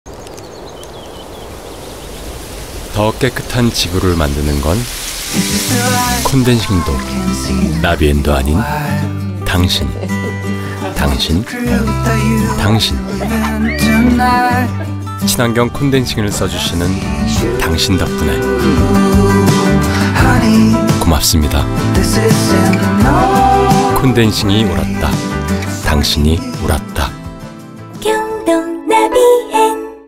[2021 경동나비엔 TV-CF] 고맙습니다 30초
더 깨끗한 지구를 만드는 건,
콘덴싱도, 나비엔도 아닌 당신
친환경 콘덴싱을 써주시는 당신 덕분에
고맙습니다